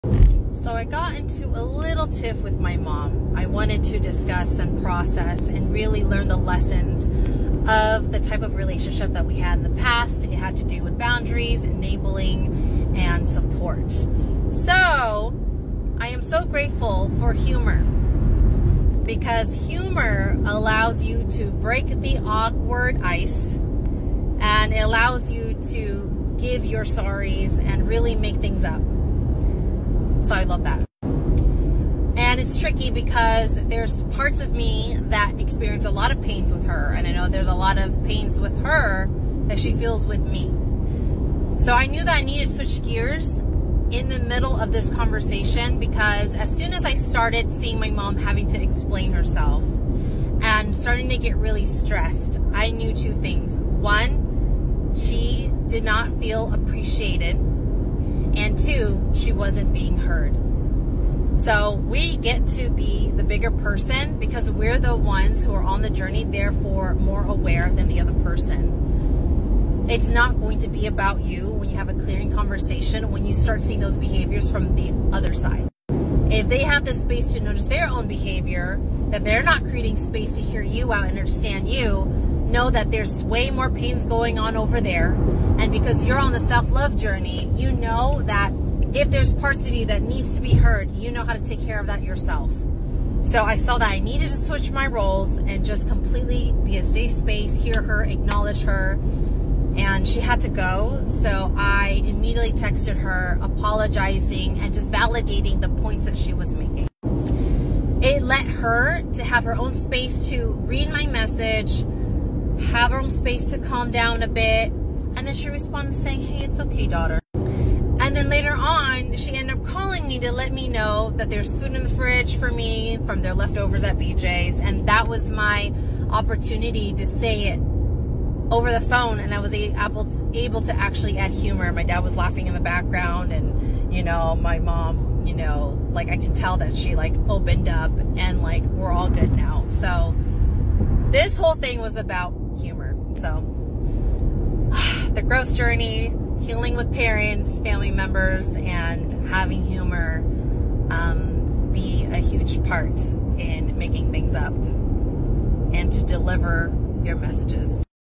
Making up to a loved one
Got into a lil tif with my mom. Here is how self-love helps with being about to making things up with your loved ones.

Thank goodness for humor, thank goodness for self-love practices that helps you create space for this AND to take the lead on being the bigger person.